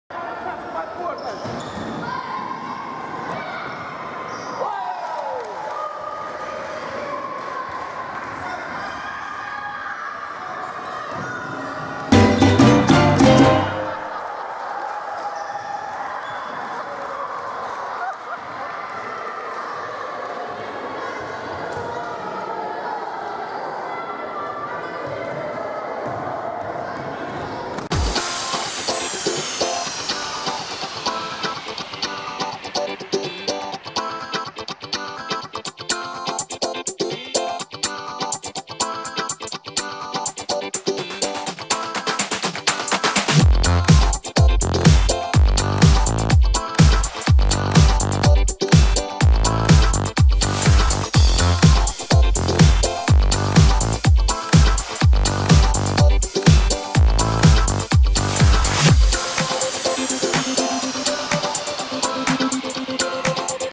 The First Buzzer Beater
The 1st 3 points Buzzer Beater in my Life
Alex Guerega #8
11 years old